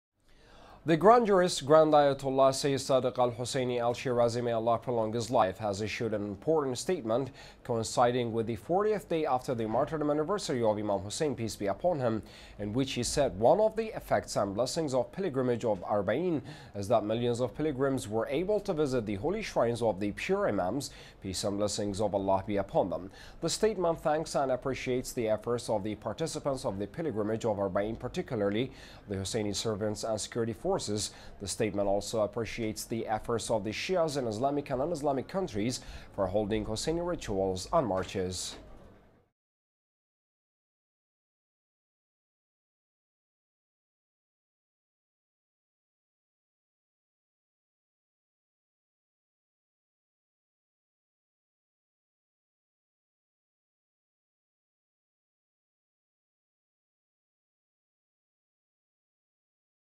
Grand Ayatollah Shirazi thanks efforts of participants in Arbaeen pilgrimage
The Grand Jurist, Grand Ayatollah SayyidSadiq al-Husseini al-Shirazi, may Allah prolong his life, has issued an important statement coinciding with the fortieth day after the martyrdom anniversaryof Imam Hussein, peace be upon him, in which he said: "One of the effects and blessings of pilgrimage of Arbaeen is that millions of pilgrims were able to visit the holy shrines of the pure imams, peace and blessings of Allah be upon them.
The statement thanks and appreciates the efforts of the participants of the pilgrimage of Arbaeen particularly the Husseini servants and security forces.
The statement also appreciates the efforts of the Shias in Islamic and non-Islamic countries for holding Husseini rituals and marches.